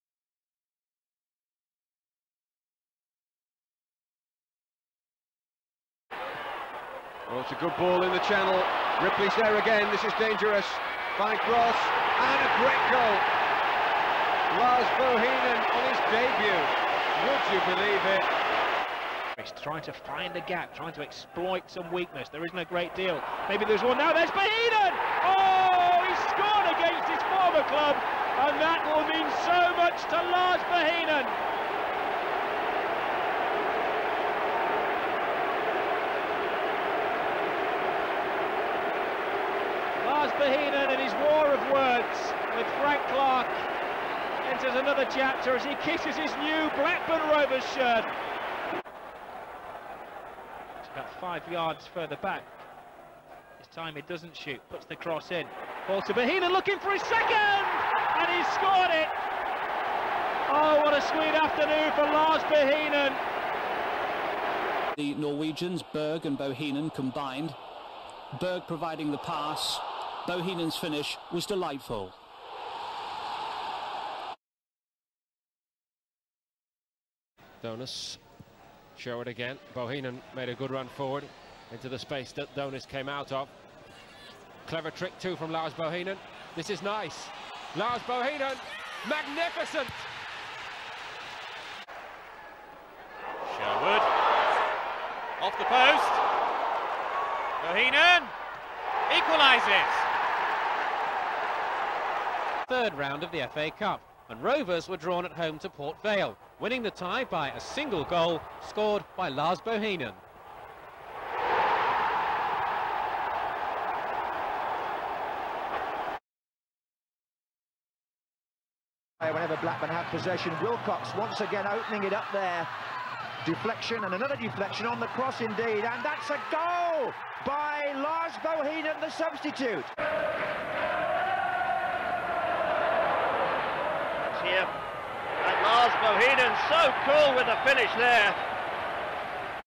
Lars Bohinen | All 9 goals for Blackburn Rovers
Norwegian midfielder, Lars Bohinen, signed for Blackburn Rovers in October 1995 after Rovers met the £700k buy out clause in his contract with Nottingham Forest.

Bohinen's decision to sign for Rovers was to the dismay of his manager at Forest, Frank Clark, who accused Bohinen of making a money orientated decision. It is believed that Rovers were to pay Lars a salary of £10k a week, whereas he was on £4k a week during his time at Forest.

Bohinen made an instant impact in a Rovers shirt as he scored on his debut in a 2-1 win against Southampton. He went on to score four League goals in his first season at the club, including two against Forest in a 7-0 demolition at Ewood Park. After the game, Bohinen hit back at former manager Frank Clark by claiming that he had signed for a better club.

One of the goals Bohinen is remembered for during his time at Rovers is his solo effort at Old Trafford in August 1996 as Rovers drew 2-2 against Man United.

Bohinen is a fond reminder of the 90's for Blackburn Rovers fans and his creative style of play is something which the club needed in the years following the title win and is something that the fans won't forget.

Making 66 appearances for Rovers and scoring 9 goals in total. Here are all 9 of Lars Bohinen's goals in a Rovers shirt.